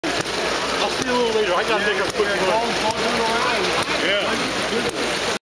Cabin Fever 2009 Model Engineering Expo - Model Steam Tractor
A Model Steam Tractor running at the Cabin Fever 2009 Model Engineering Expo